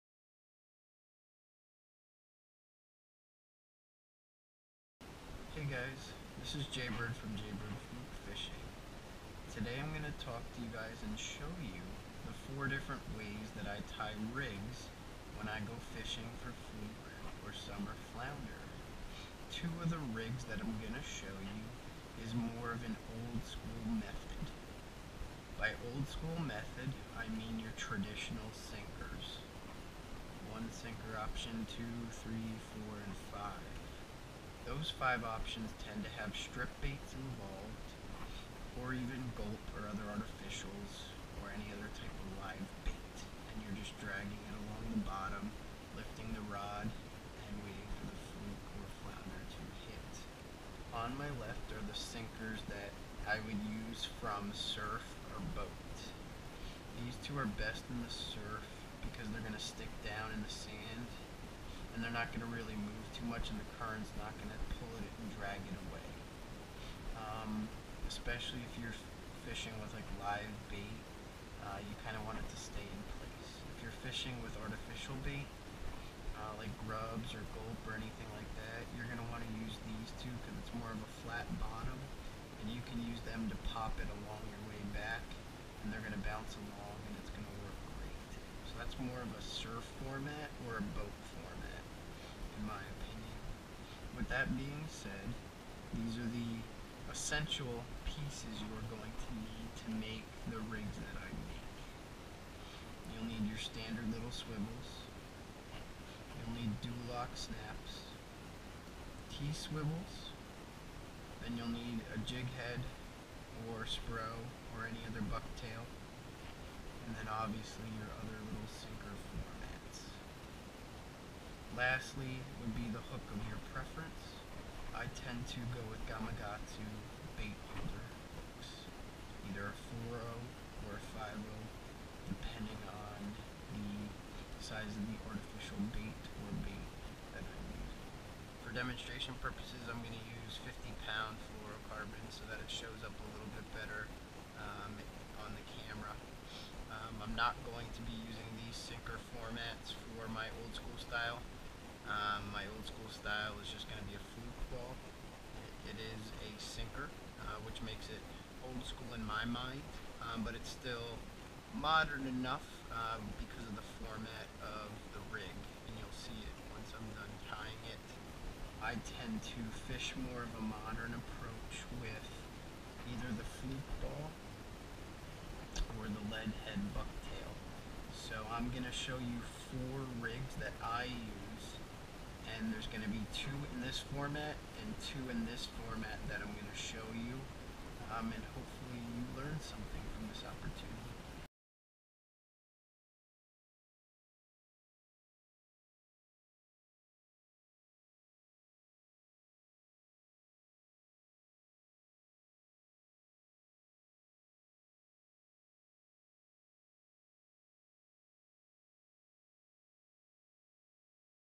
RIG DISCUSSIONS FOR FLUKE/FLOUNDER
A RIG DISCUSSIONS VIDEO THAT WILL HELP YOU DISCOVER WHAT KIND OF RIGS YOU NEED FOR FLUKE/FLOUNDER FISHING